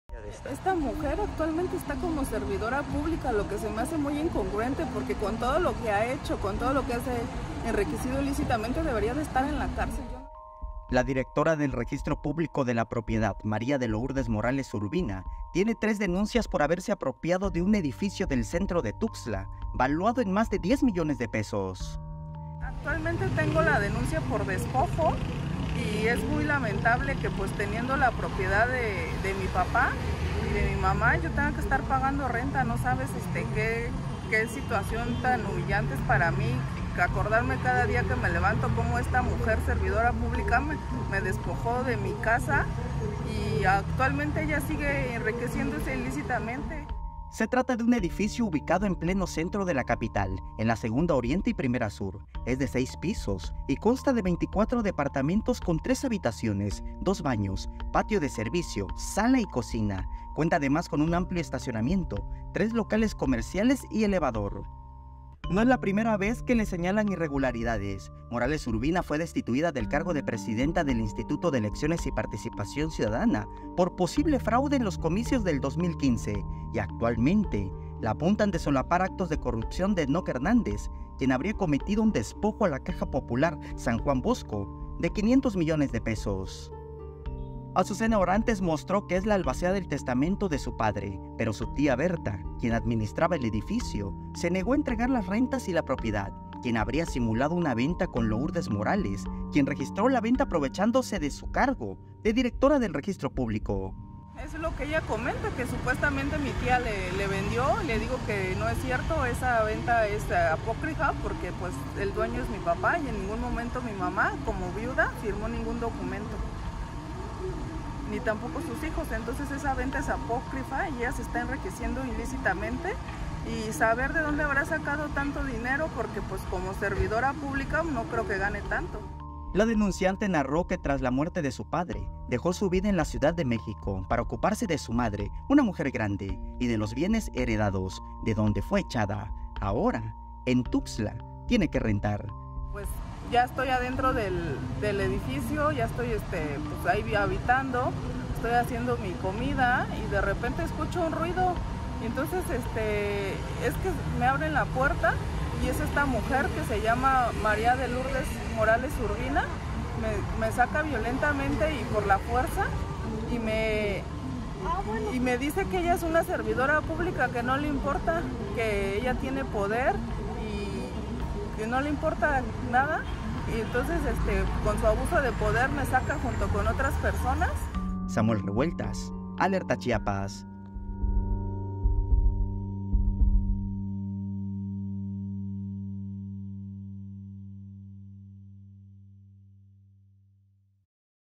Se apropia directora del Registro Público de un edificio en el centro
La directora del Registro Público de la Propiedad, María de Lourdes Morales Urbina, tiene tres denuncias por haberse apropiado de un edificio del centro de Tuxtla valuado en más de 10 millones de pesos.